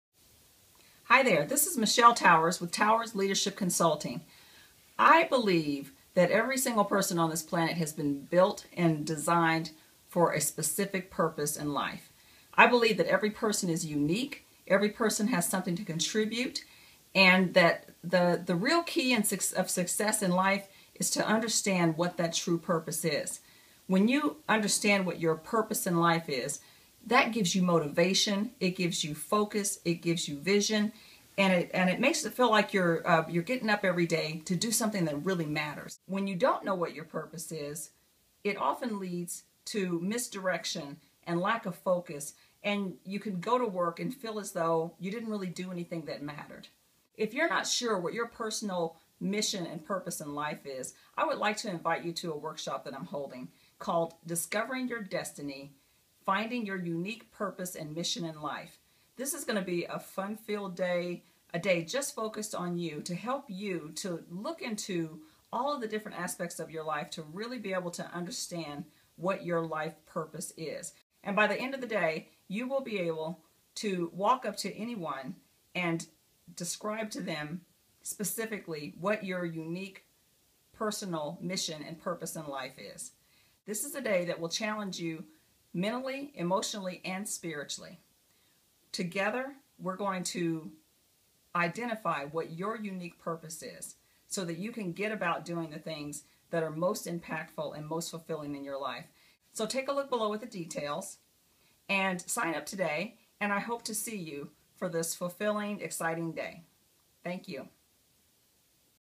"Discovering Your Destiny: Finding Your Unique Purpose and Mission in Life" Workshop
Are you in tune with your life purpose and mission?

Understanding your life mission can mean the difference between living a life of fulfillment, or one that lacks direction or passion.  If you are not sure of your life purpose, here is what you can do to find it!